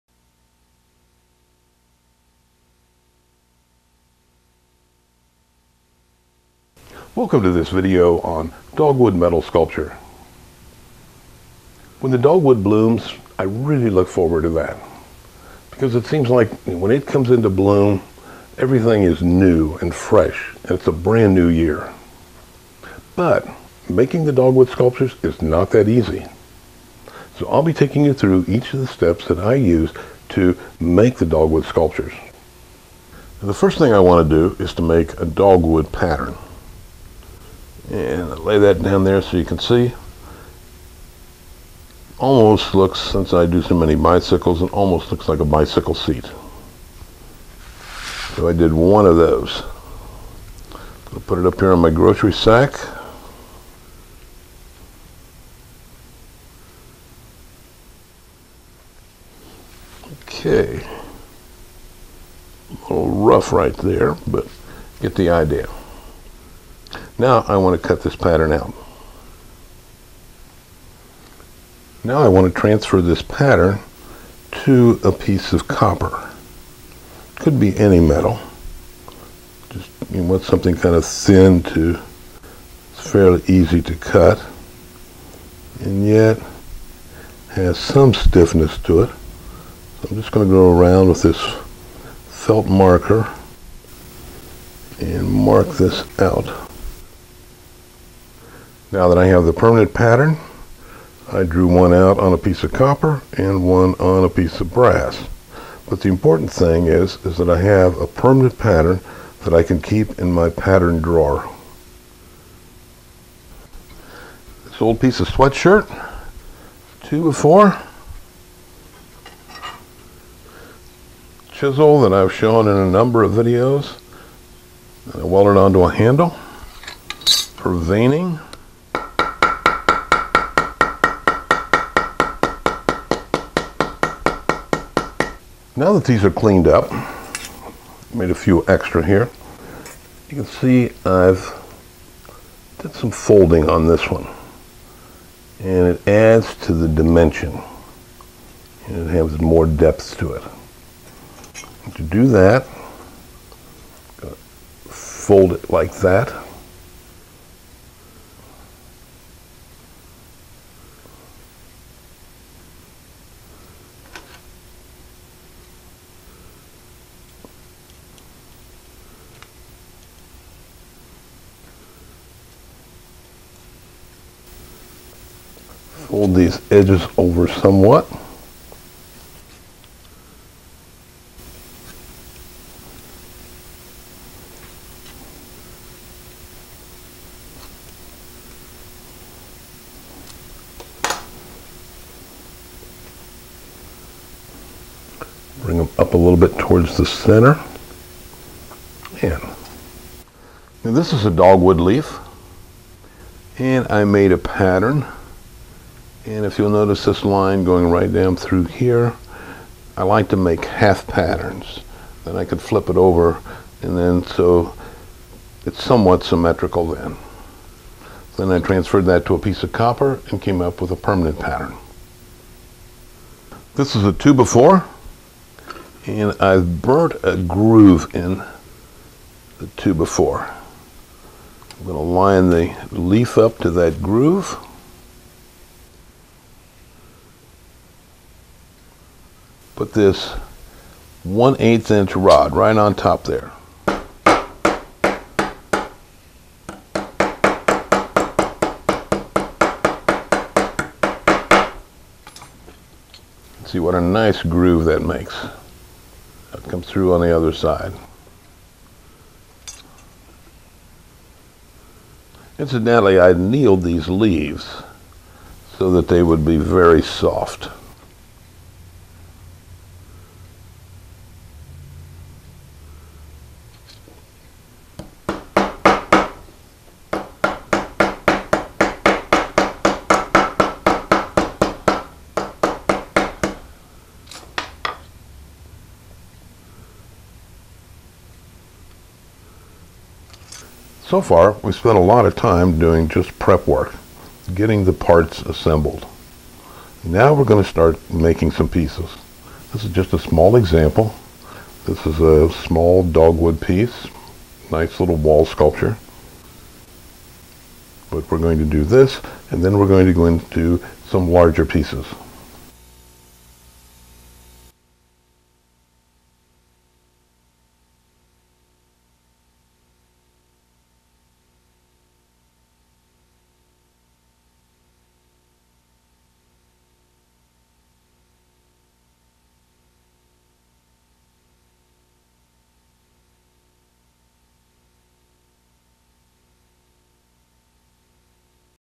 Dogwood Metal Sculpture
Preview of the Dogwood Metal Sculpture DVD.  Included in this preview is making the pattern for a dogwood blossom and the leaves.  Once you have the basic steps down you can build a small dogwood spray right up to a large dogwood spray.

I don't find these particularly easy.  They always sold well in my shop and I could have sold more, but sometimes I get lazy.   The main point is that people like dogwood in any form.